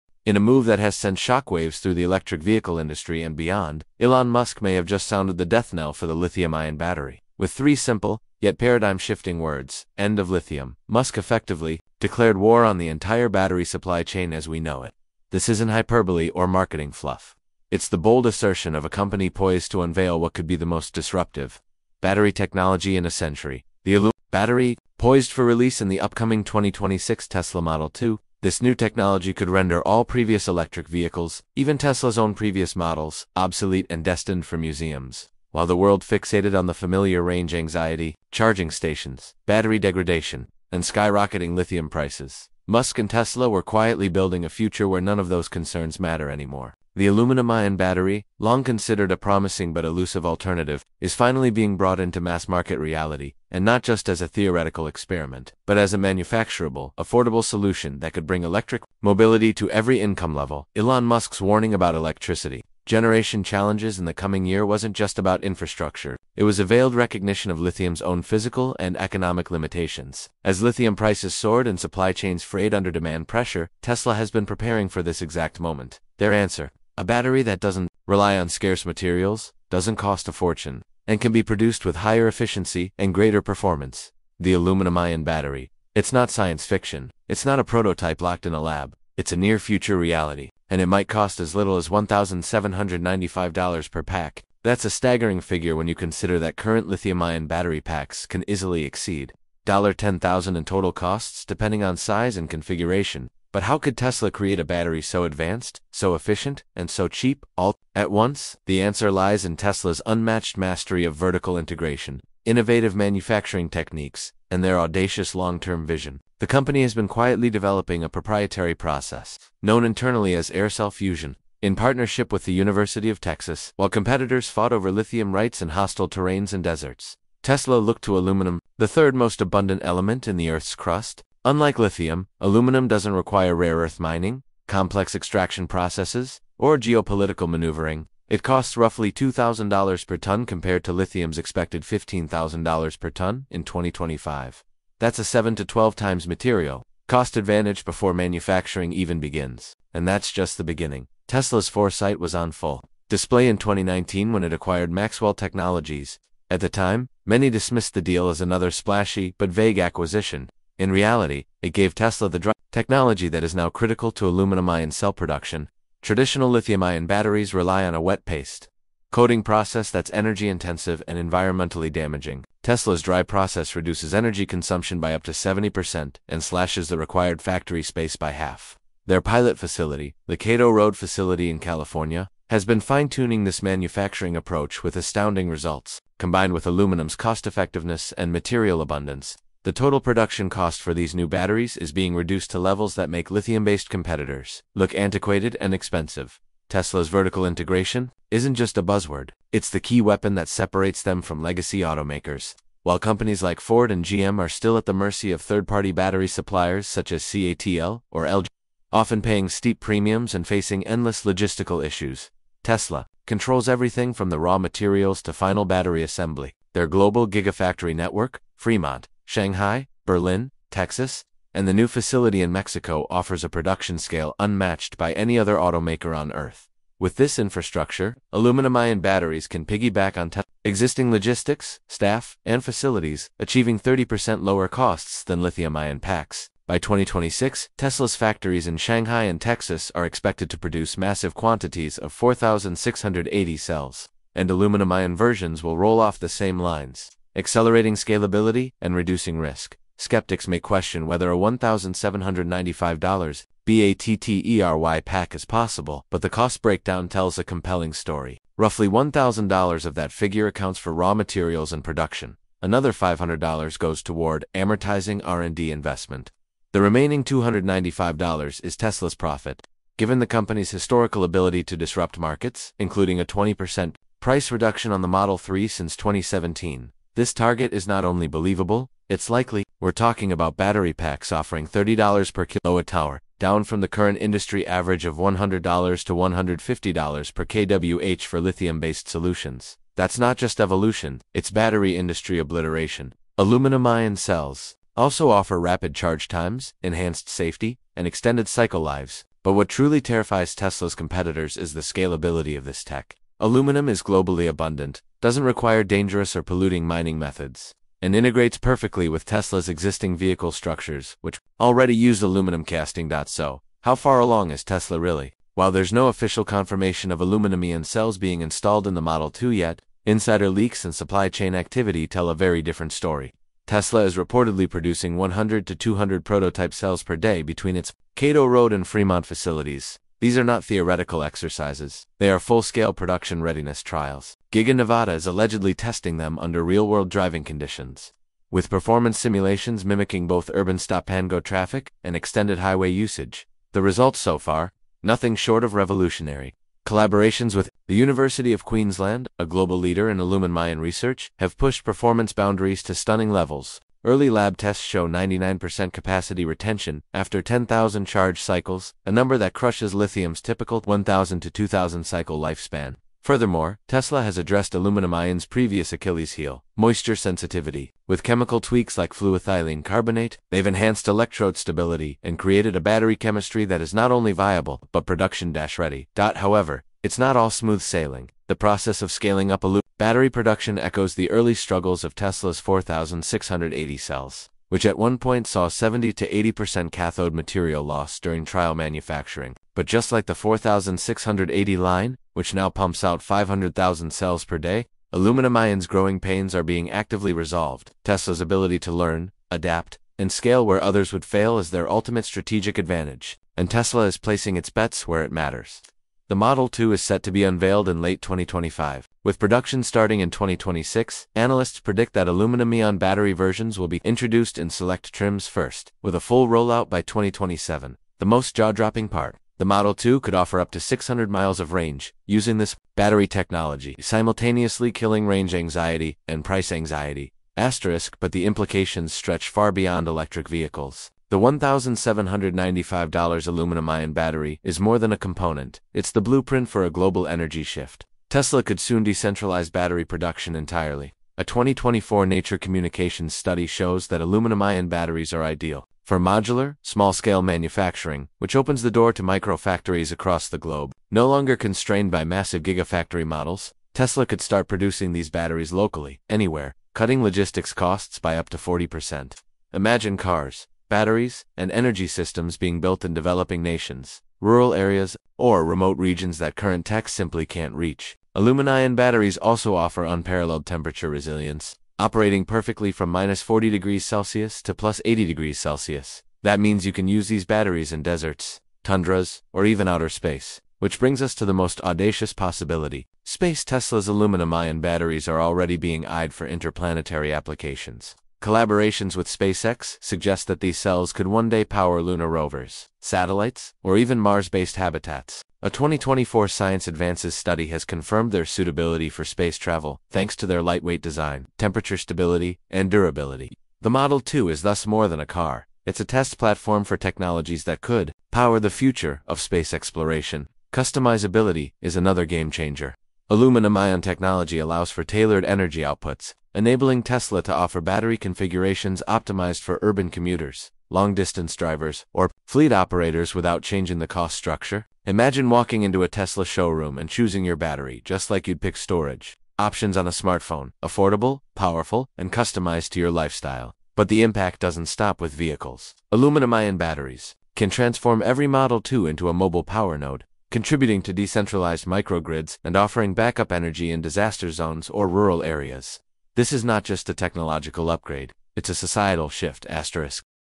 Elon Musk Reveals $1,795 Aluminum-Ion Battery for 2026 Tesla Model 2, Ending Lithium's Reign
🚨 Major Battery Breakthrough Alert! 🚗⚡

Elon Musk has just announced a game-changing shift in EV technology: the upcoming 2026 Tesla Model 2 will feature a revolutionary Aluminum-Ion battery, priced at just $1,795. This bold move could mark the end of the lithium-ion era, reshaping the entire electric vehicle industry.

The new battery promises:
✅ Faster charging times
✅ Longer lifespan
✅ Improved safety
✅ Lower production costs
✅ Eco-friendliness with less reliance on rare or environmentally harmful materials

If successful, this breakthrough could make EVs more affordable, efficient, and sustainable than ever before. The Aluminum-Ion battery also positions Tesla to lead in next-gen battery innovation, potentially disrupting not only the auto industry but also energy storage globally.